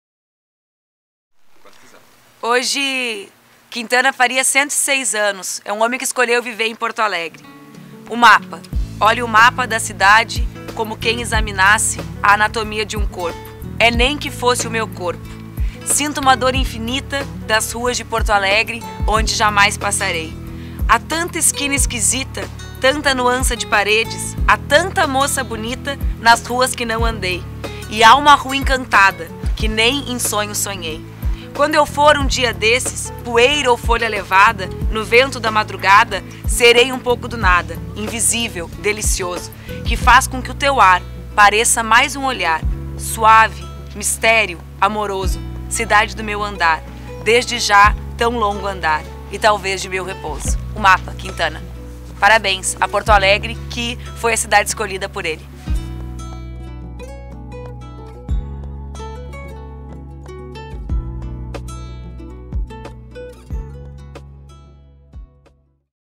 Mario Quintana 106 anos
Mário Quintana completaria 106 anos neste 30 de julho de 2012.
Escolhi o poema "O Mapa" para homenagear essa pessoa que escolheu Porto Alegre como casa.  Obrigada, Mário Quintana.